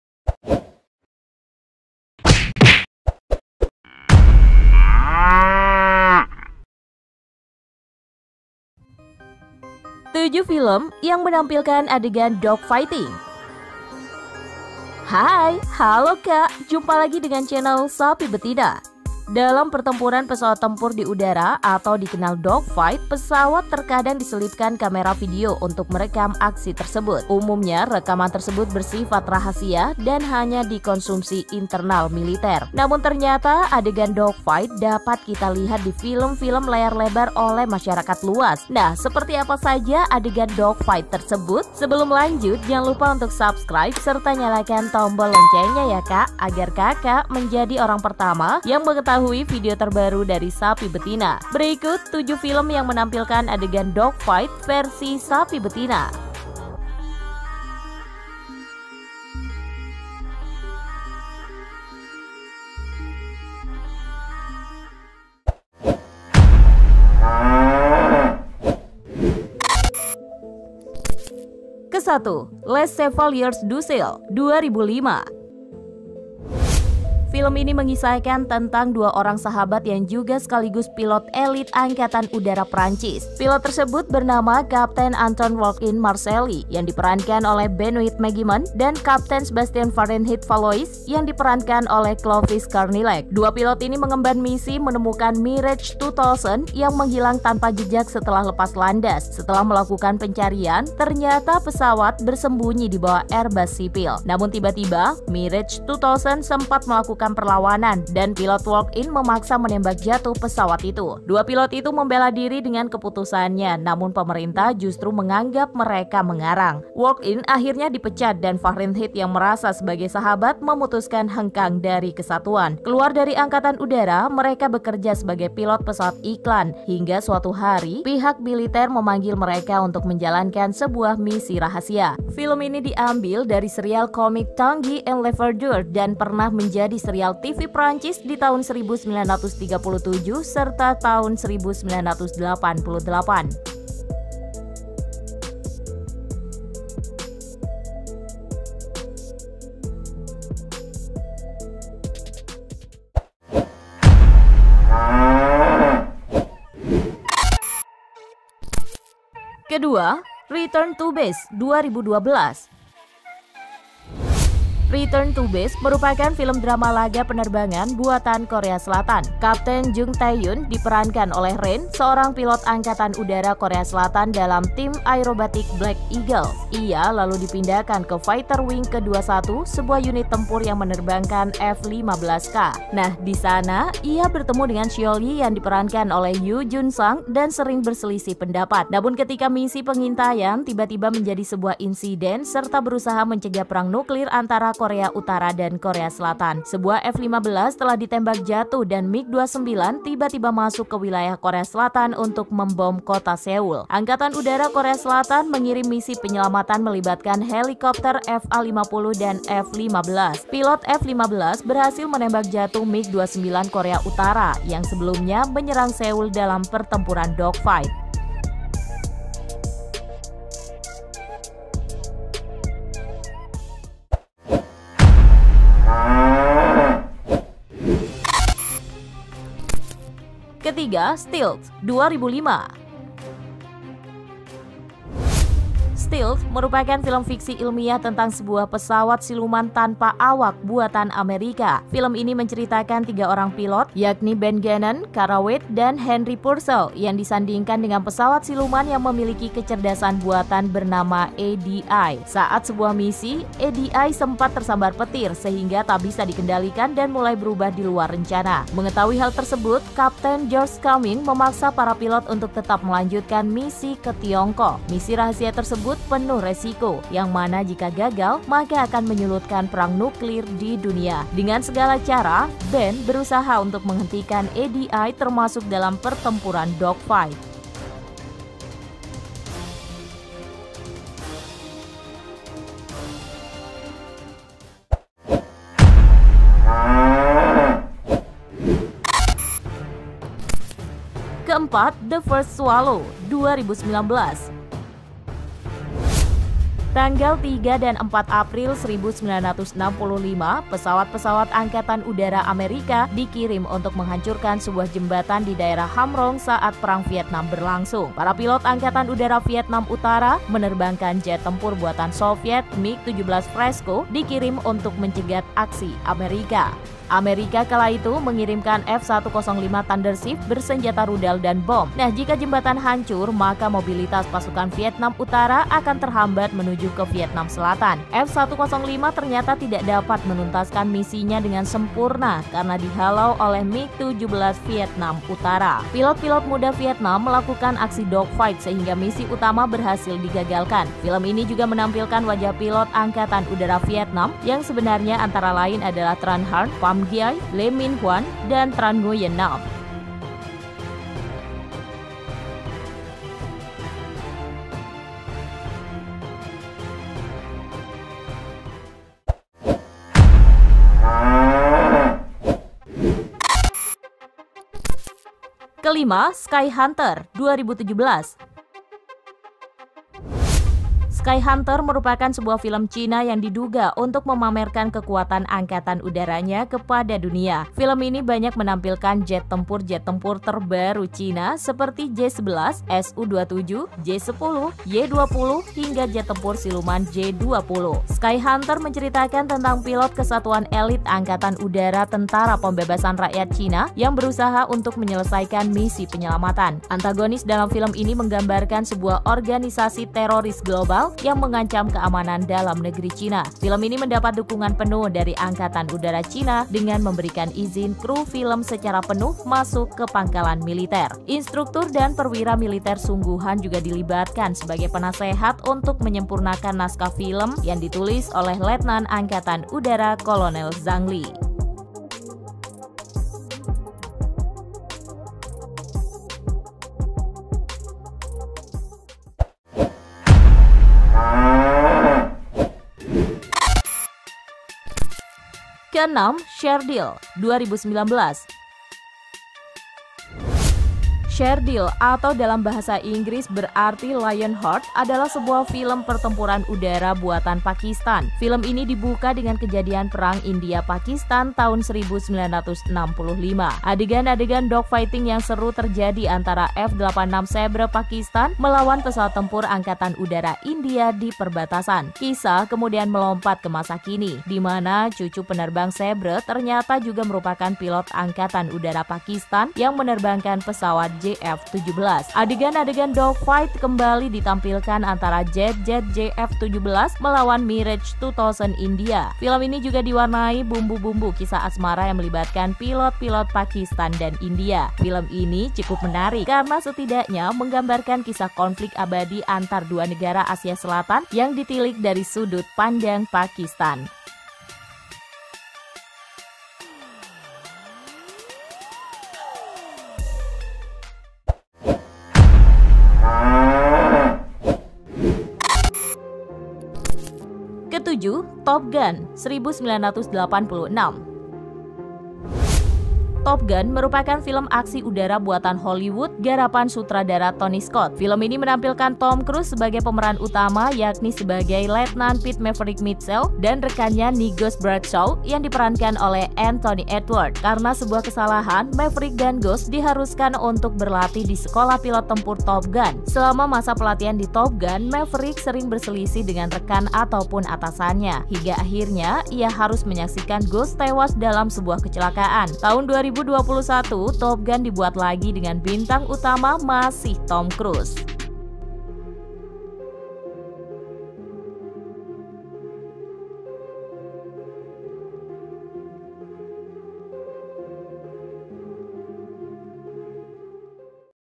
SANGAT SERU! Deretan Film yang Menampilkan Adegan Pertempuran Dogfight
Dalam pertempuran pesawat tempur di udara atau dikenal Dogfight, pesawat terkadang diselipkan kamera video untuk merekam aksi tersebut.
Umumnya rekaman tersebut bersifat rahasia dan hanya dikonsumsi internal militer.
Namun ternyata adegan Dogfight dapat kita lihat d film-film layar lebar oleh masyarakat luas.
Seperti apa saja adegan dogfight tersebut?